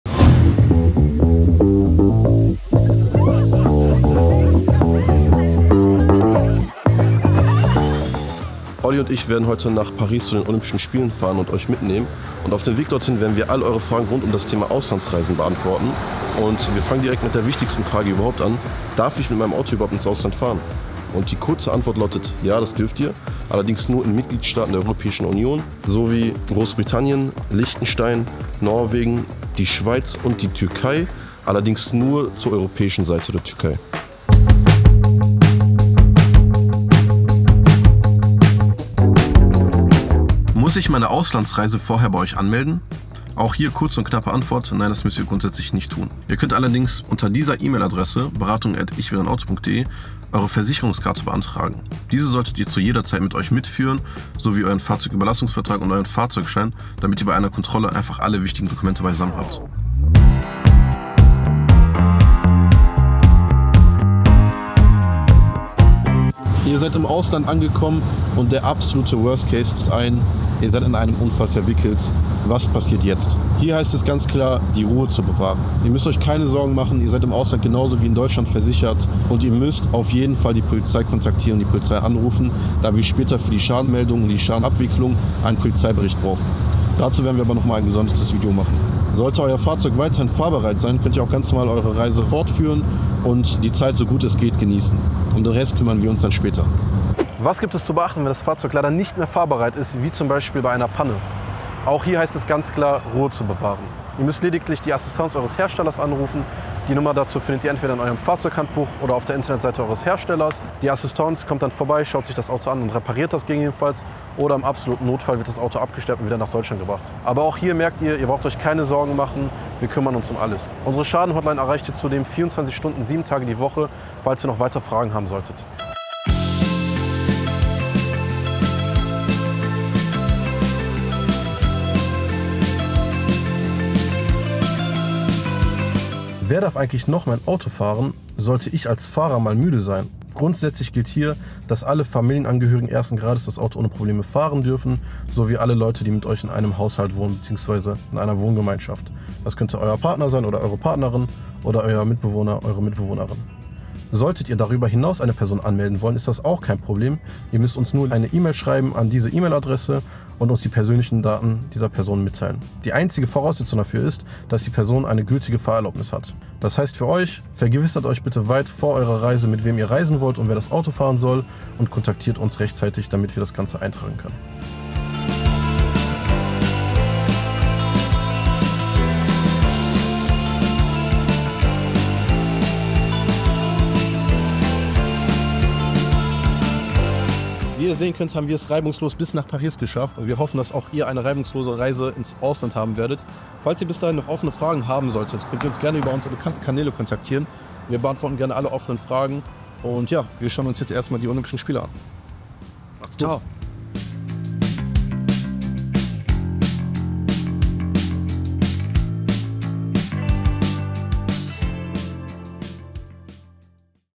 Darf ich mit dem Auto ins Ausland? 🚙 🇫🇷
Das Auto-Abo für den Sport, Bundeswehr & Beamte seit 1997.
Eine monatlich Rate, alles ist drin!

00:00 - Darf ich mit dem Auto ins Ausland fahren?
00:39 - Auslandreise anmelden? / Versicherungsschein
01:06 - Unfall im Ausland, was tun?
01:37 - Panne im Ausland
02:16 - Wer darf mein Auto fahren?
03:08 - Outro